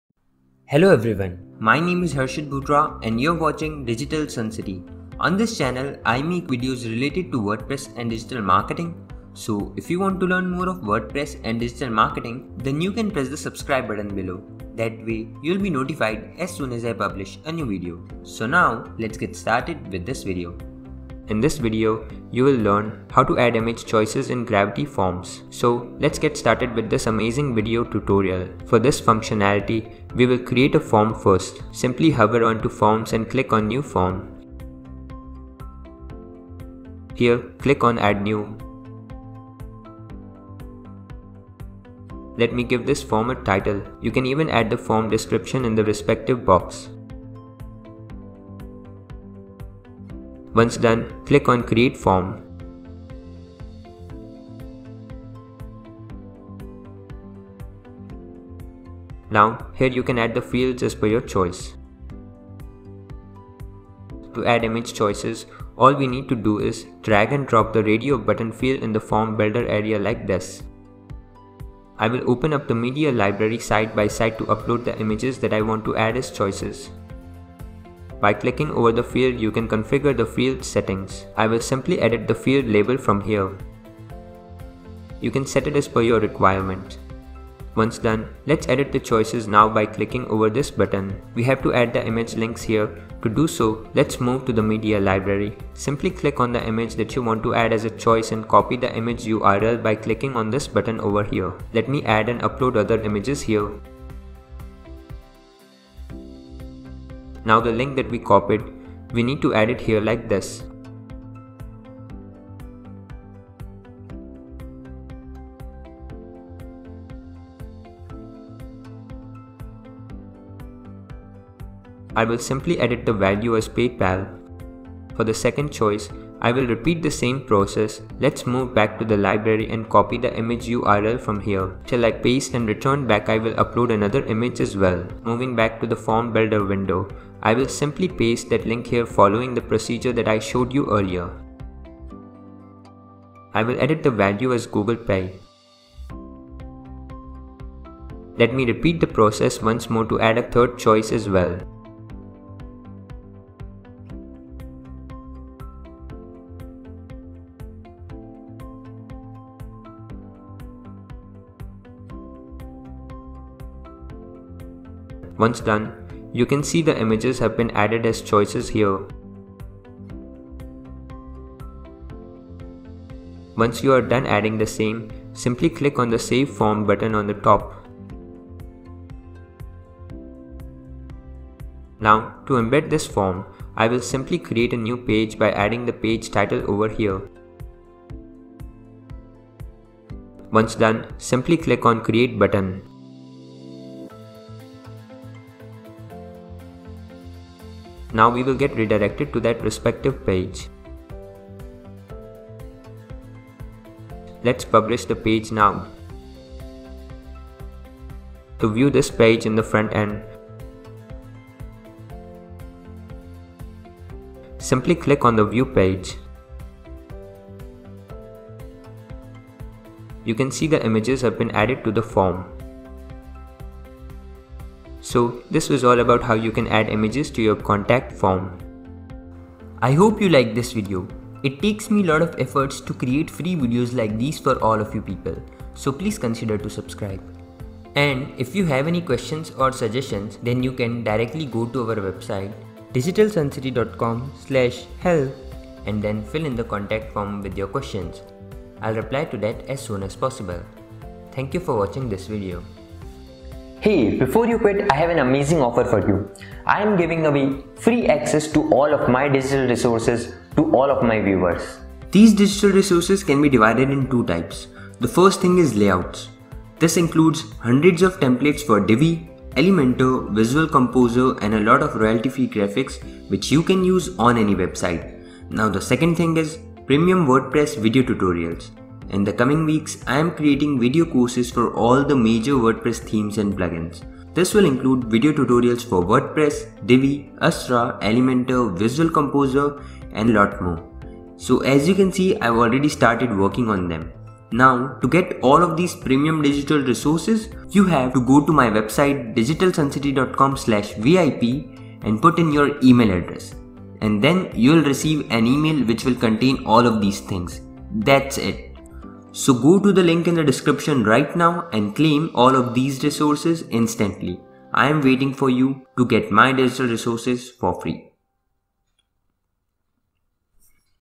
How To Add Image Choices In Contact Forms In WordPress | Gravity Forms | WordPress Tutorial
Do you want to add image choices in contact forms in WordPress?🤔

►In this video, you’re going to learn how to add image choices in contact forms in WordPress. This is a great way to make your contact forms more interactive and engaging for your visitors. It allows them to select an image that represents their inquiry or request, making it easier for you to understand and respond to their message.

►So, whether you're a blogger, a business owner or a website developer, this tutorial will help you create a more user-friendly contact form on your website.